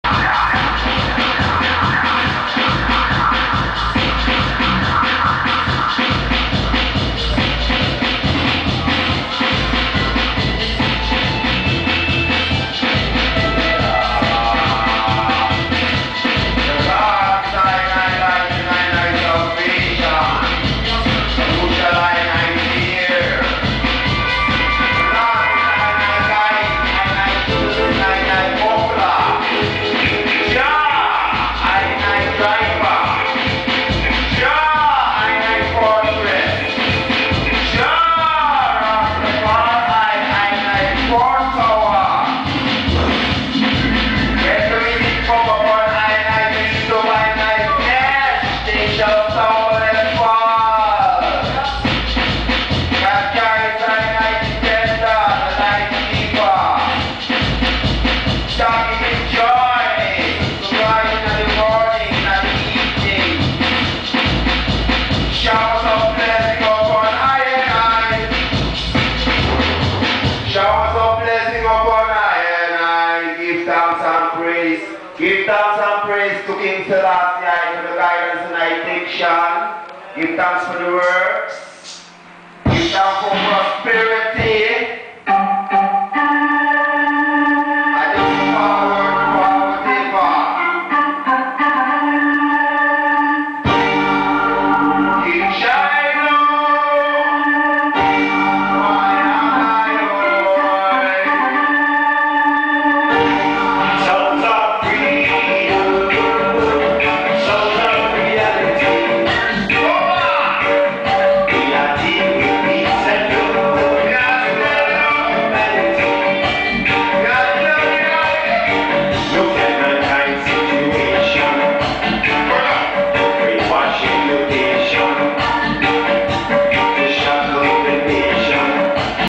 King Shiloh Israel Ze'elim Kibbutz, Soundcheck
King Shiloh Sound System outta Amsterdam on tour in Israel. First session on May 23rd 2K7 in Ze'elim Kibbutz (south). Red Lion does a soundcheck...